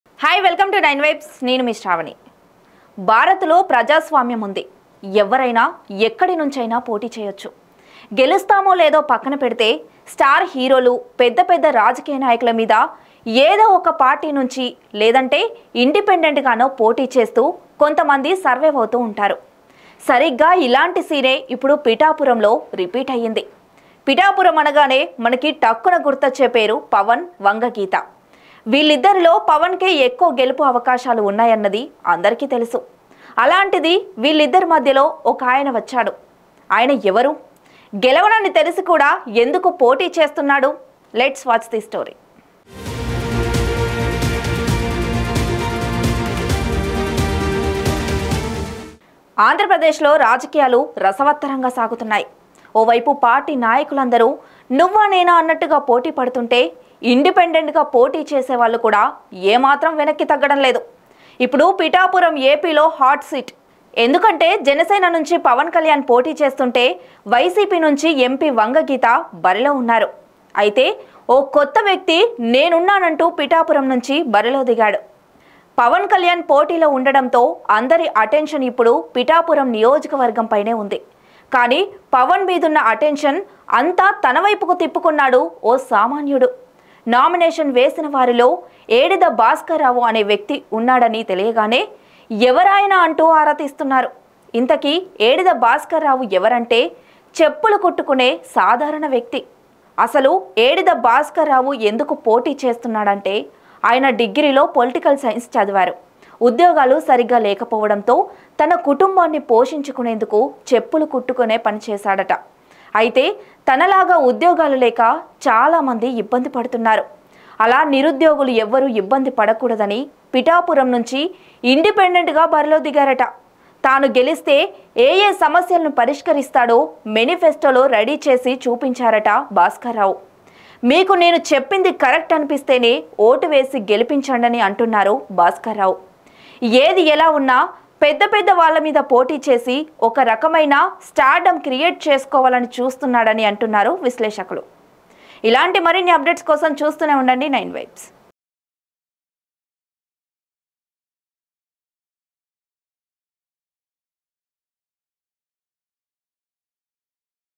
పానకంలో పుడక?Pithapuram constituency political dynamics || Pithapuram constituency political contest
In the bustling political landscape of Pithapuram, unexpected contenders emerge, challenging established figures like Pawan Kalyan and MP Vanga Geetha. Meet Edida Bhaskara Rao, a shoemaker turned political aspirant, who aims to represent the voice of the common man. In this story of unlikely heroes and political adventures, discover why Bhaskara Rao's candidacy has stirred curiosity and debate.